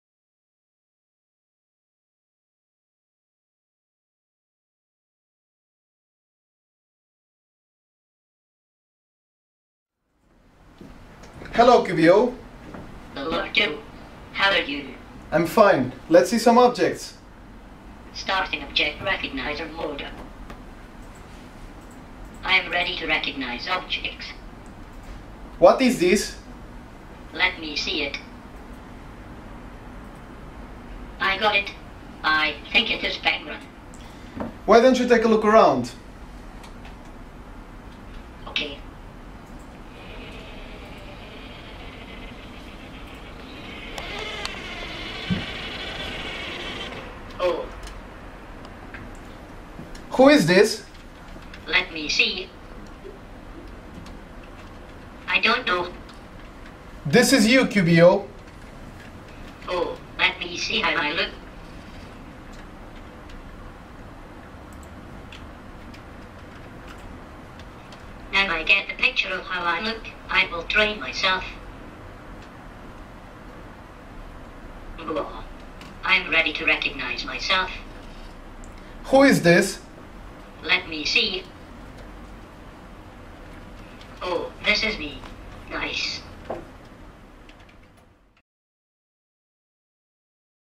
Qbo Robot Sees Its Own Reflection and Recognizes It
The Qbo robbot, by creator Francisco Paz is able to use recognition software in conjunction with pre-programmed responses, to recognize its own image.

In the video, Qbo transits to  the "Object Recognition" state from its internal state machine,  to learn to recognize its mirror image, as if it were a regular object. Technically, how does Qbo do it?

The "Object Recognition" state is implemented to execute simultaneously different ROS nodes: one responsible for the head and base movement; another to select an object from the image using the stereoscopic vision; another to recognize objects or learn new ones. The object recognition algorithm uses "SURF" descriptors and the "Bags of Words" approach (through  the OpenCV library), and stores images in Qbo's internal storage system.

Qbo has several stored answers and behaviors in an internal knowledge base, that we upgrade as the projects evolves, to make questions or orders to Qbo such as "What it this? or "Do this". Qbo interprets the object "Myself" as a an ordinary object, for which it has special answers in its internal knowledge base such as "Woah. I'm learning myself" or "Oh. This is me. Nice". Qbo selects its reflection in the mirror in the image that he sees using the stereoscopic vision, and one of our engineers interacts (speaks) to him so that Qbo can learn to recognize himself as another object. For direct interaction, Qbo uses the open-source software Julius for speech recognition (in the video, you see how Qbo receives the order to turn around, and he responds to it by moving its base 90 degrees), and Festival for voice synthesis.